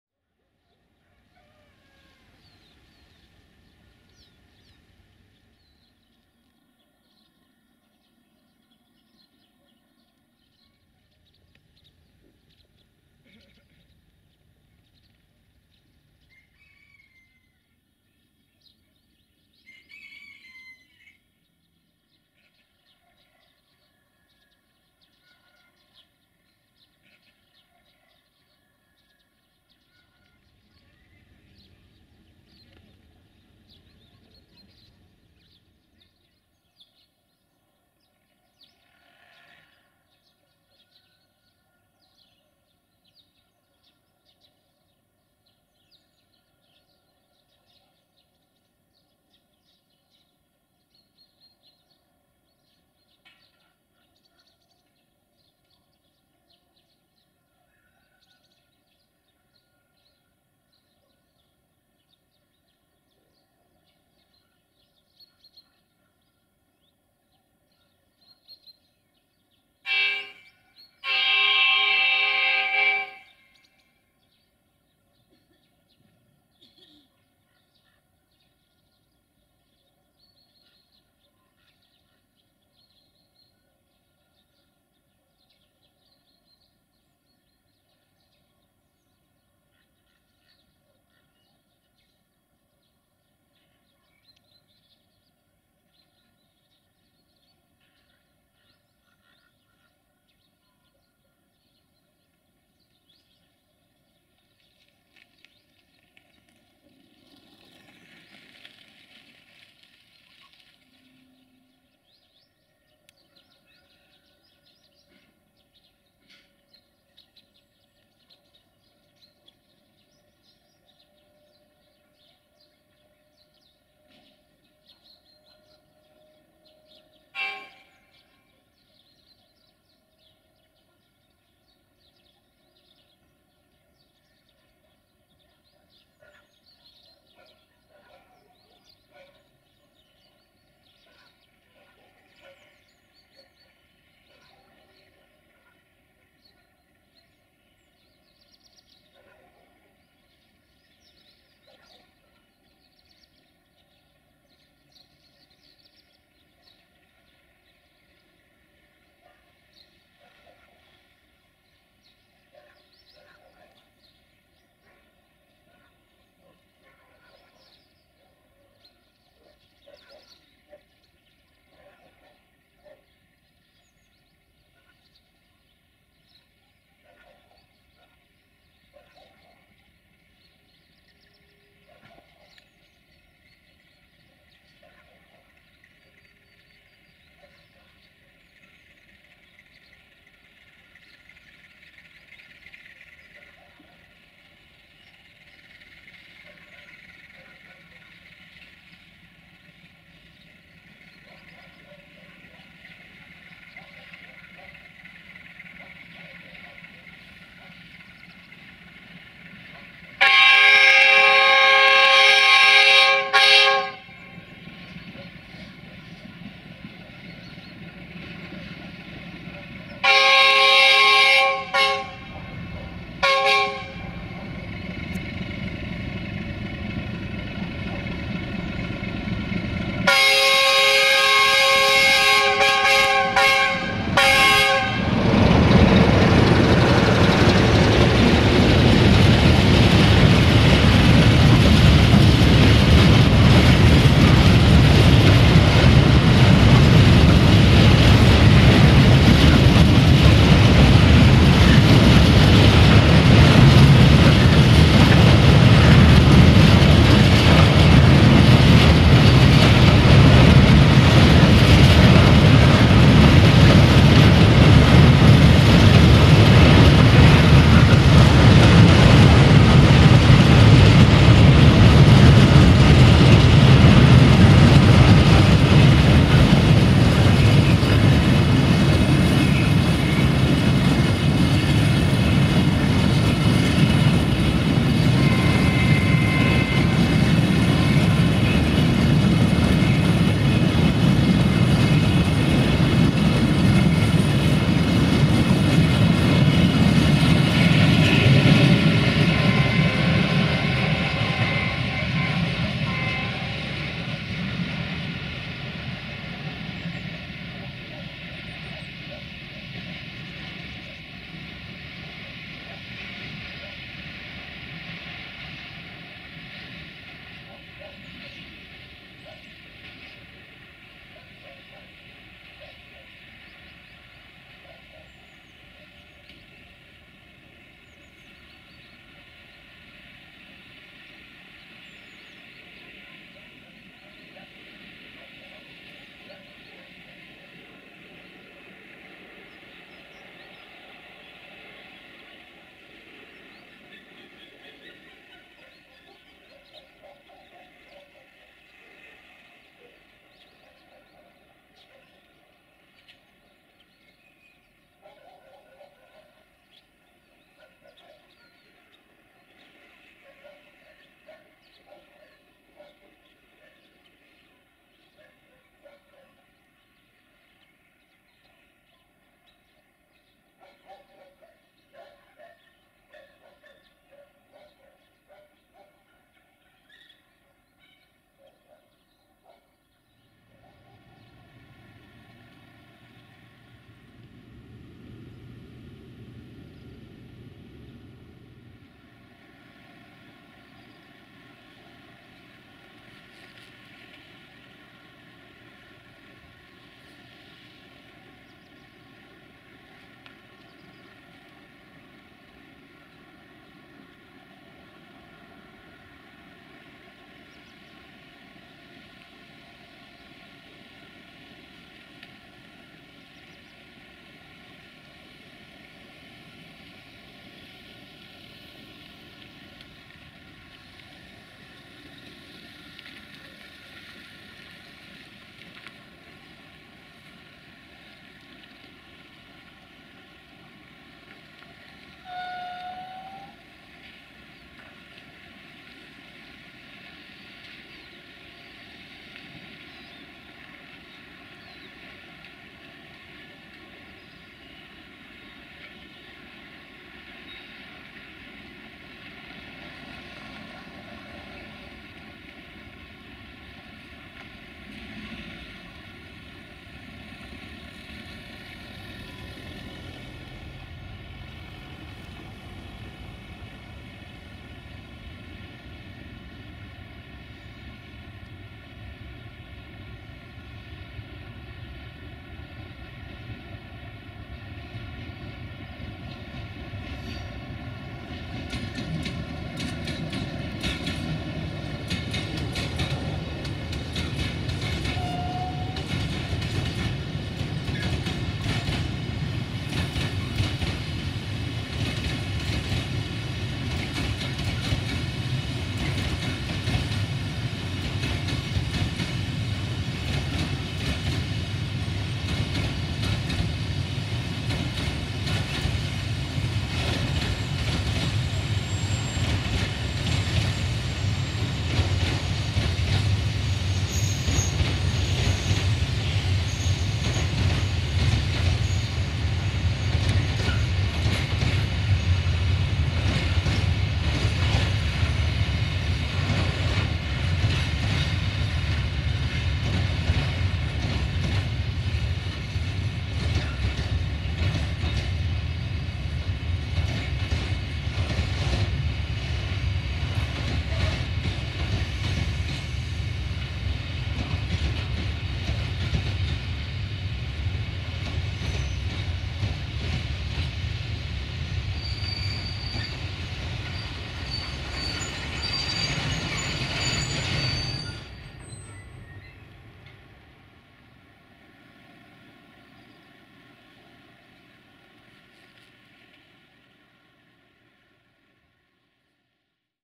LDE2100 60-0434-0 & Mecanic Super Engineer in Gara Valea lui Mihai Station - 15 October 2020
00:38 Prva Slovenska Zeleznicna Freight Train/Marfar/Guterzug - 060-DA/LDE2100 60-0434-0 PSZ/RO-BEU
06:30 CFR MARFĂ Freight Train/Marfar/Guterzug - 060-DA/LDE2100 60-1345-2 RO-SNTFM

15 October 2020